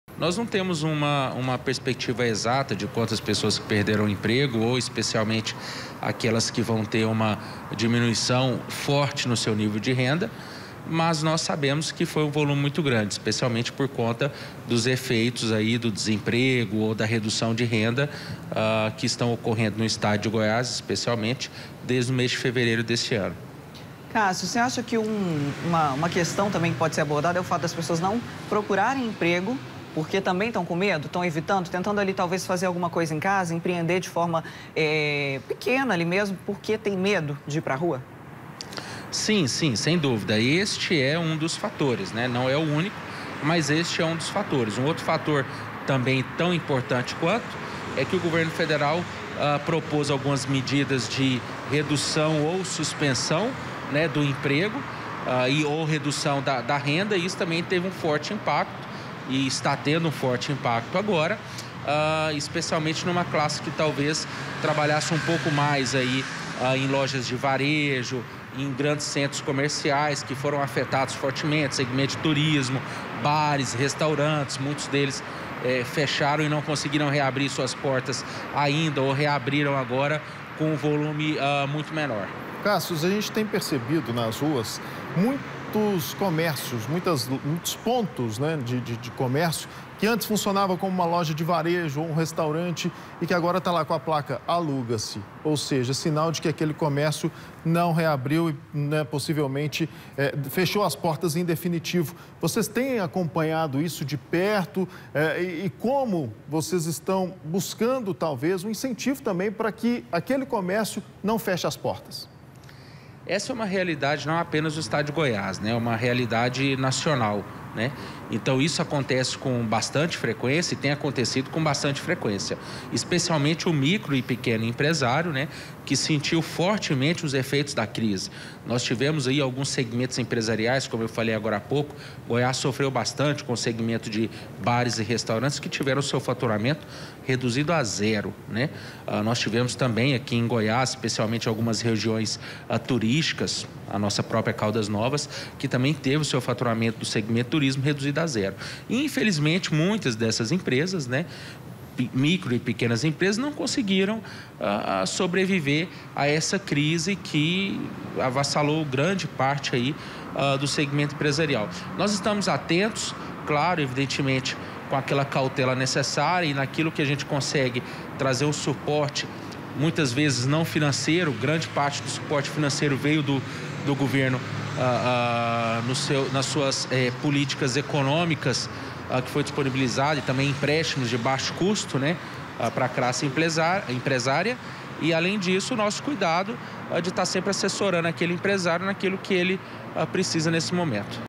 PANDEMIA PODE AUMENTAR O NÚMERO DE PESSOAS NAS CLASSES MAIS BAIXAS
UM ESTUDO MOSTRA QUE POR CAUSA DA PANDEMIA DEVE AUMENTAR O NÚMERO DE PESSOAS NAS CLASSES MAIS BAIXAS. 15 MILHÕES DE BRASILEIROS PASSARÃO A TER UMA RENDA ABAIXO DE 2 MIL E 500 REAIS PARA FALAR SOBRE ESSE ASSUNTO NÓS VAMOS CONVERSAR AGORA COM O CONSULTOR FINANCEIRO DA ACIEG, CASSIUS PIMENTA.